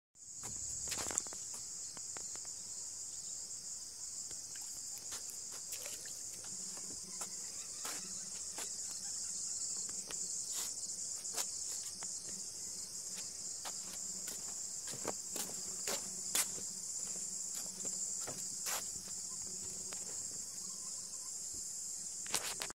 Walleye Diver | Homemade Lure Action.
This is the action of my homemade short ! What are your thoughts?